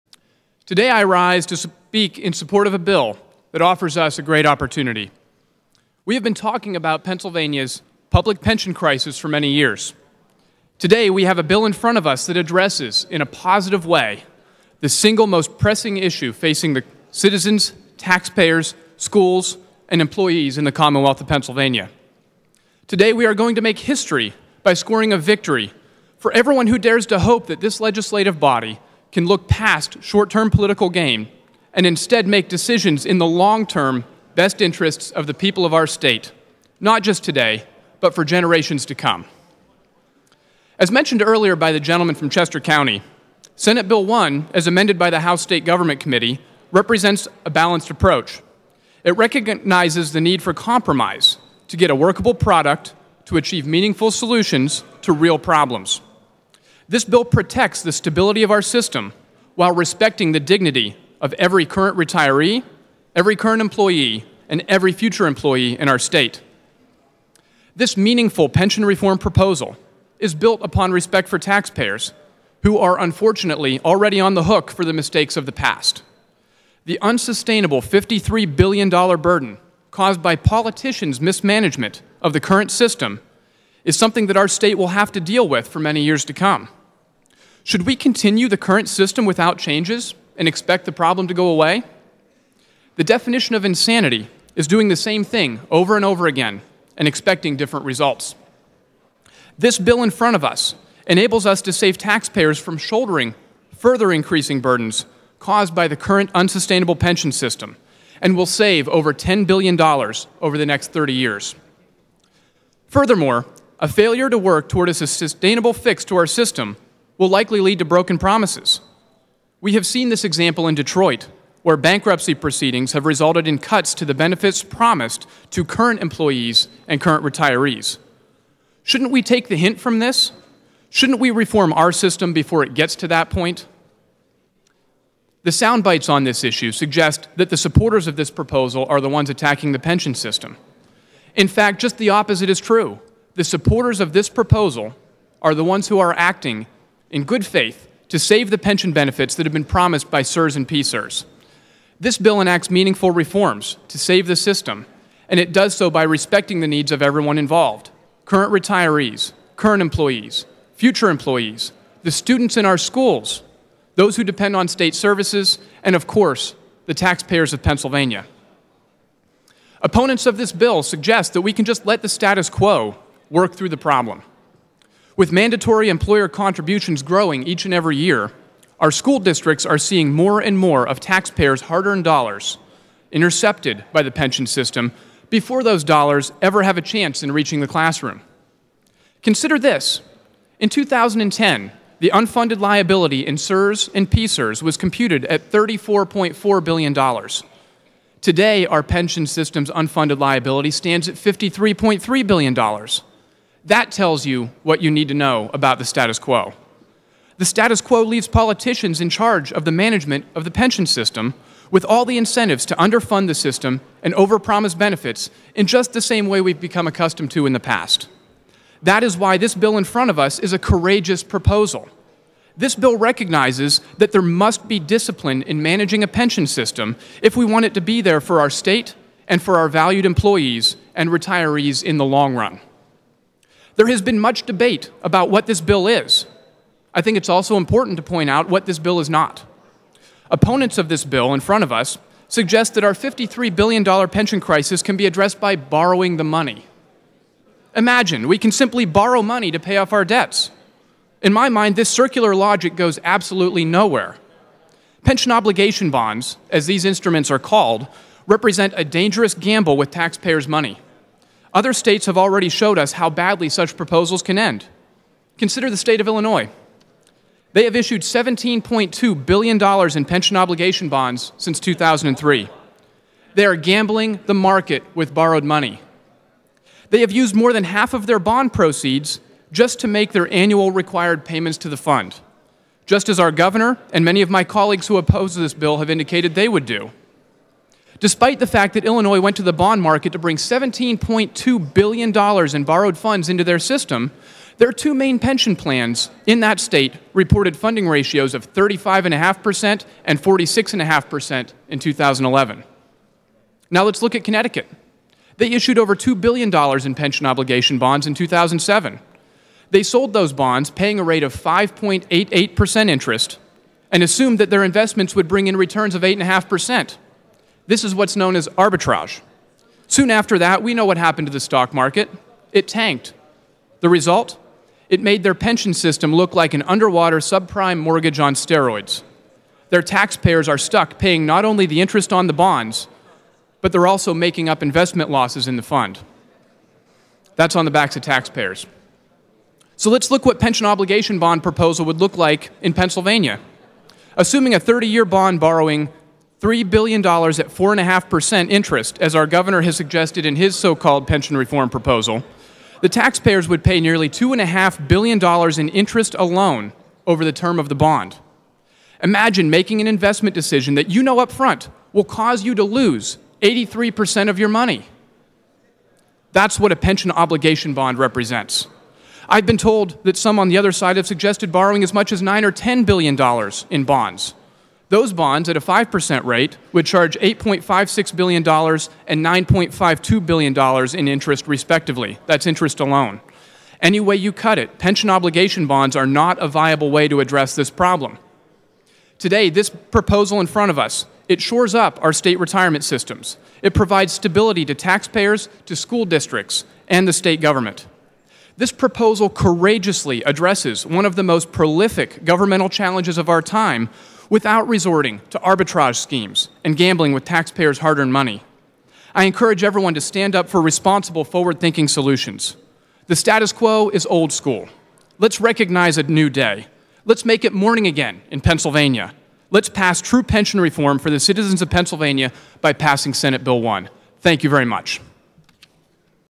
Meaningful Pension Reform
PA State Rep Matt Gabler presents his insight into SB 1, the Pennsylvania pension reform bill and how it offers a balanced and compromising approach toward fixing a broken system.